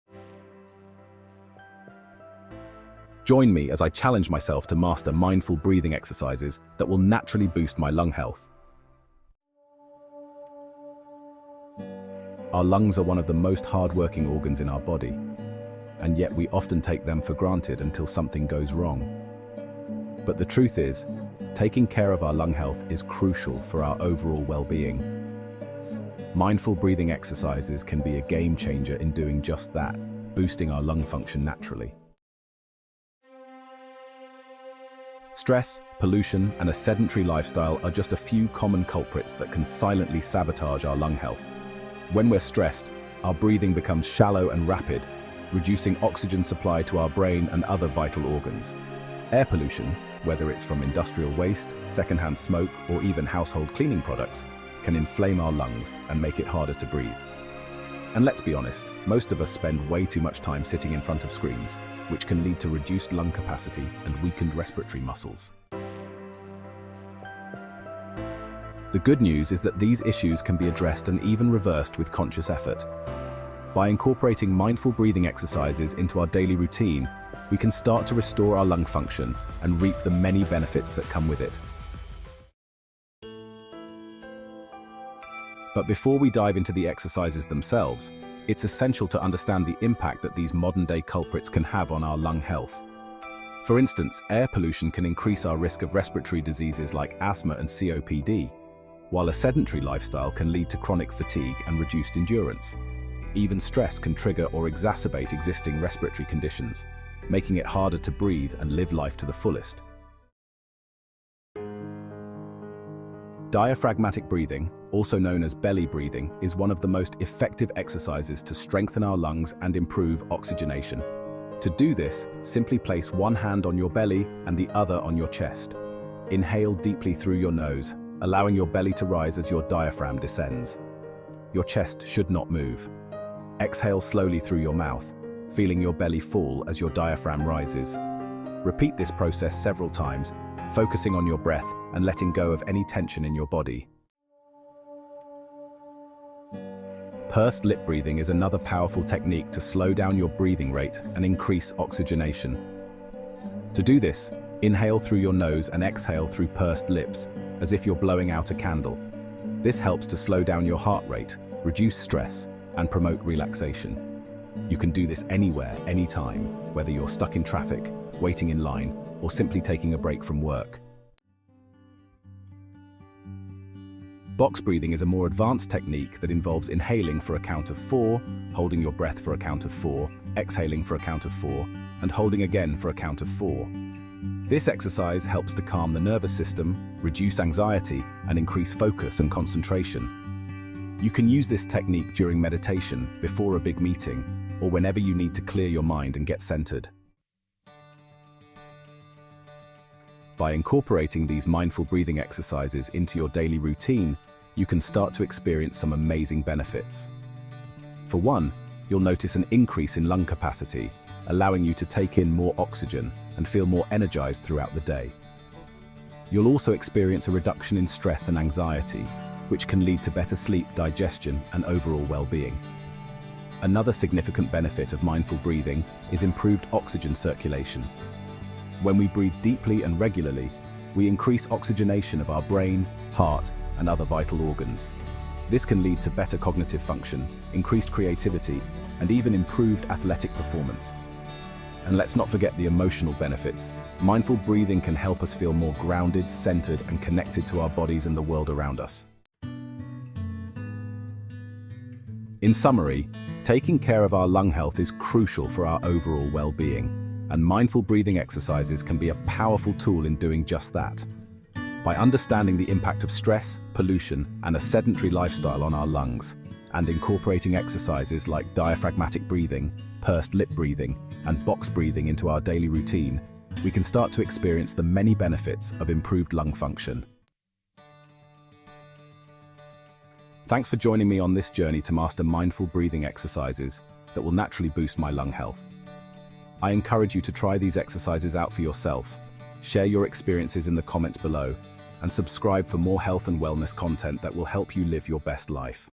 2024: Why breathing exercises are CRITICAL for lung health
Mindful Breathing Exercises to Boost Lung Health Naturally

Learn how to improve your lung health naturally with these mindful breathing exercises.

This video will guide you through effective breathing techniques that can help increase lung capacity, promote relaxation, and boost overall respiratory function.

Practice these exercises regularly to experience the benefits of better lung health. Breathe easy and take control of your respiratory well-being today!